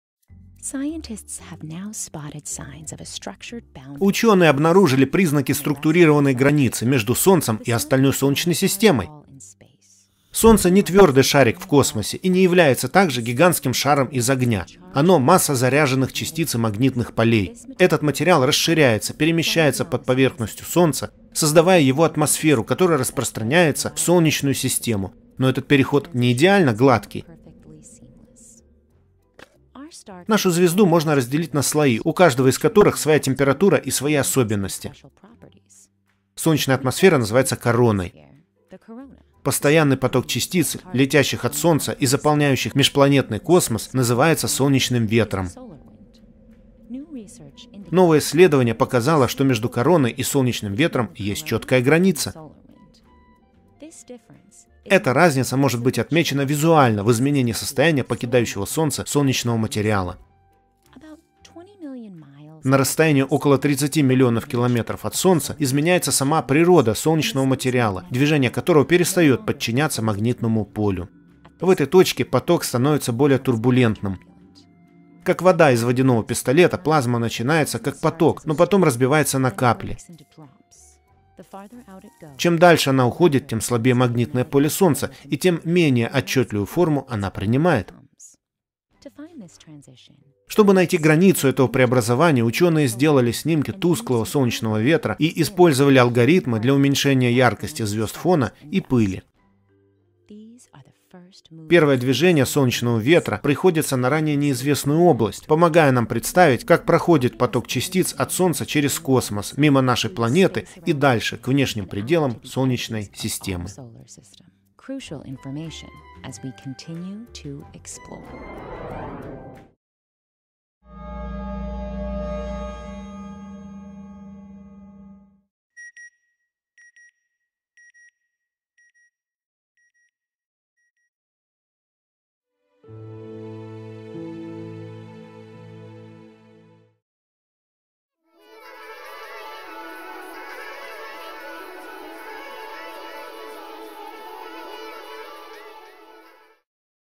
NASA | Гелиофизика | Снимки границы Солнца
Ученые обнаружили признаки структурированной границы Солнца!